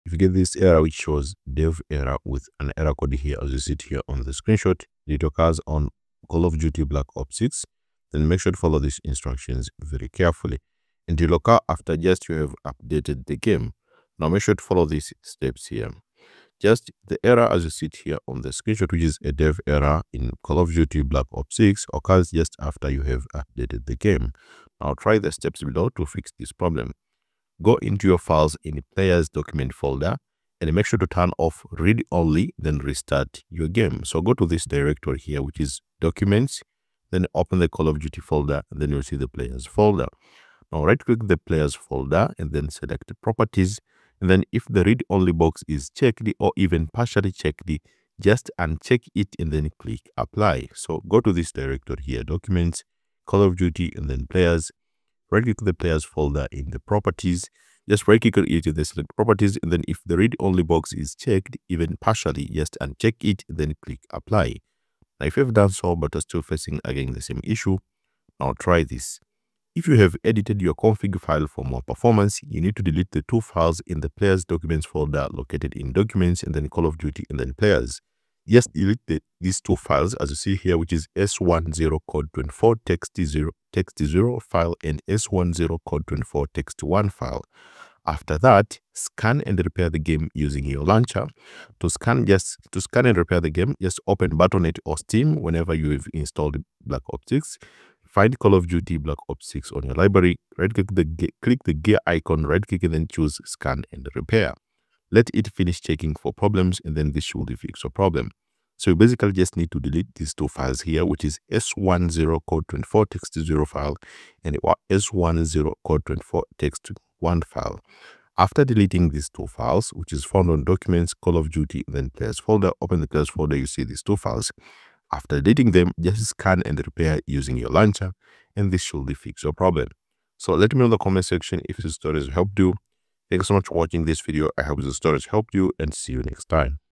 Fix DEV ERROR 0x872394DC in Call of duty: Black ops 6
The error DEV ERROR 0x872394DC in COD black ops 6 occurs just after you have updated the game

Try the steps below to fix This problem

Go into your files in players document folder and make sure to turn off read only then restart your game
Go to:Documents → Call of Duty → players
Right-click the players folder → Properties
If the Read-only box is checked (even partially), uncheck it.
Click Apply

If it Fails try the steps below
if you have edited your config file for more performance you need to delete the two files in the Players documents folder located in
Documents\Call of Duty\players
Just delete this two files
 s.1.0.cod24 TXT0 File and s.1.0.cod24 TXT1 File
After that scan and repair the game using your launcher